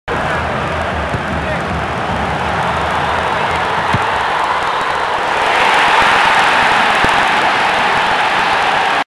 SweetPatch Goal of the Month March 09 - Birch Tree 07
Here is a sick chip through pass and volley goal by Birch Tree 07